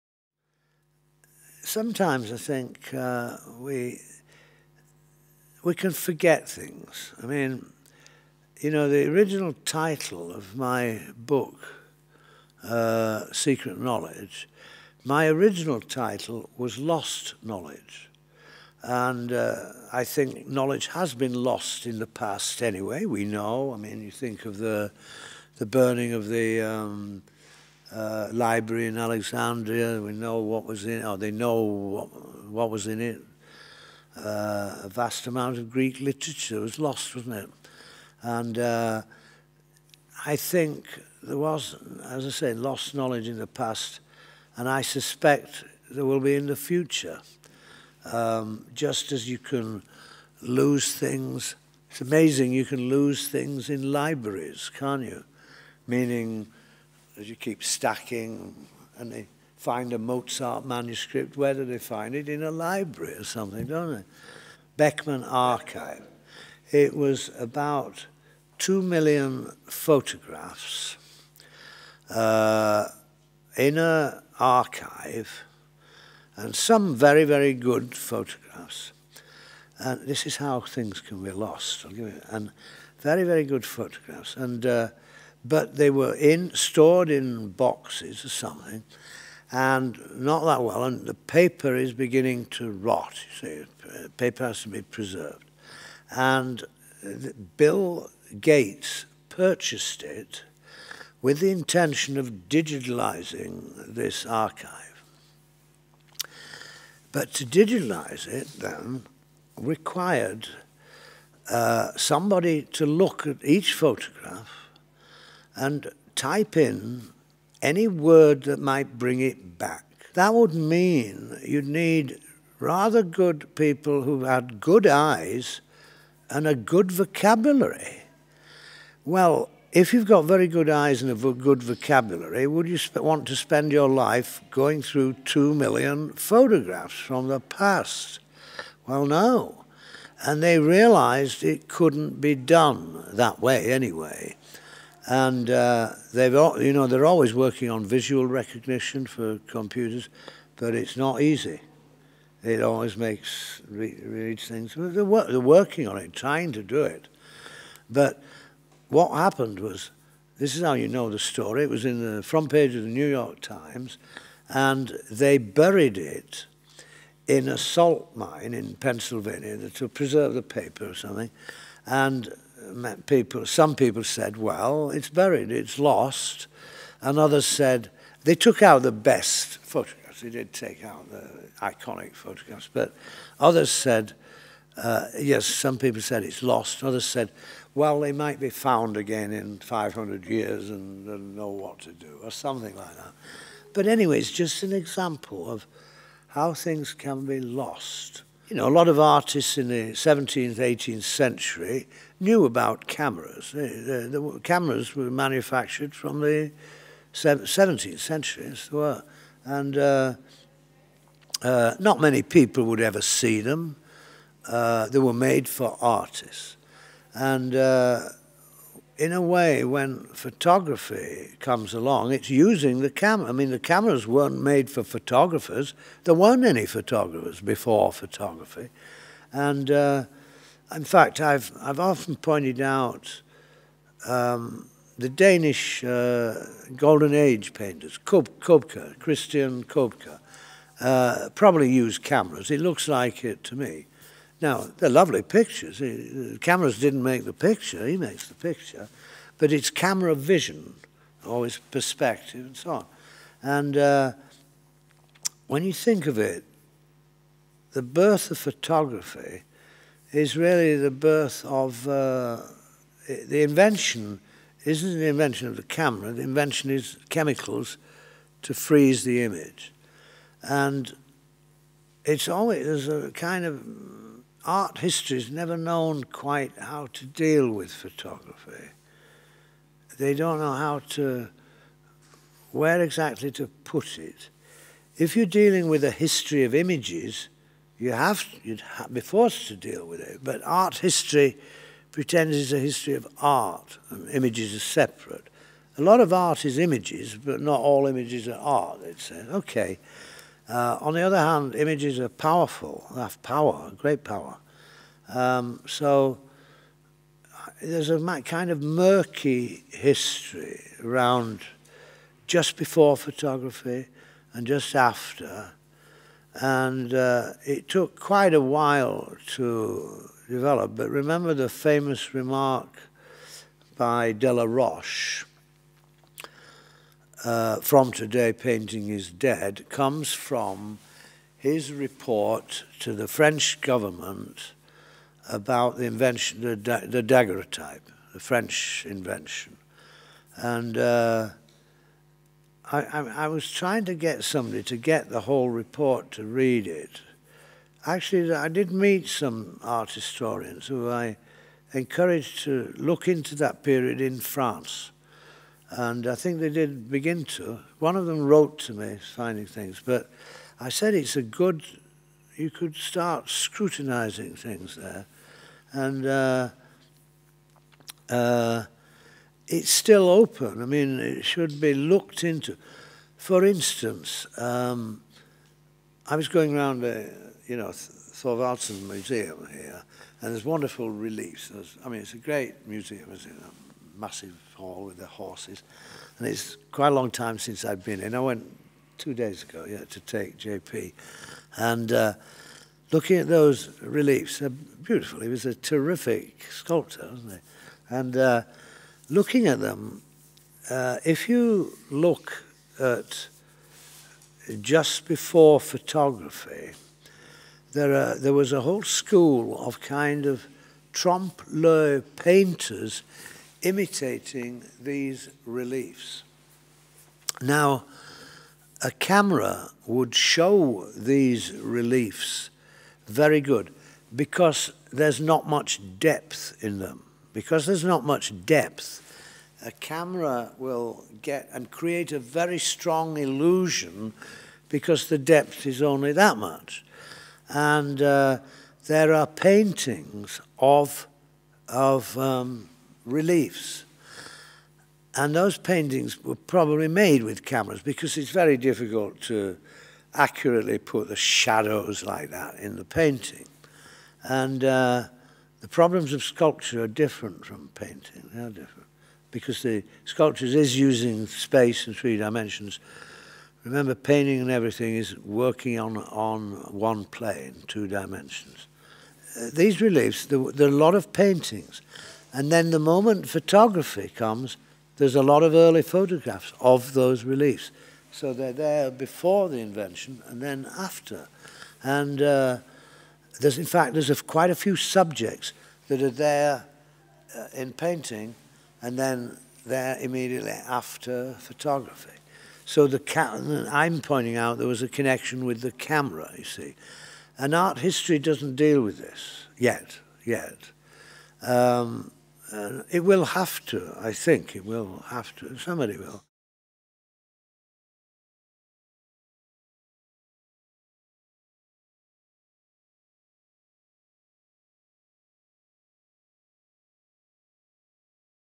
David Hockney Interview: Lost Knowledge
British artist David Hockney talks about the hidden role of photography in art history and the problem of preserving human knowledge in the digital age.

David Hockney (b. 1937) is one of the most important contemporary artists living today. In this interview he talks about the birth of photography and its "murky" role in art history. A theme that he deals with in his groundbreaking book "The secret knowledge". "Art history has never known how to deal with photography" he says, and points out how the "camera vision" has influenced art and the artistic process.

David Hockney was interviewed by Christian Lund, Louisiana Museum of Modern Art, March 2011 in connection with the ’Me Draw On iPad’ exhibition.

Produced by: Martin Kogi and Christian Lund
Copyright: Louisiana Channel, Louisiana Museum of Modern Art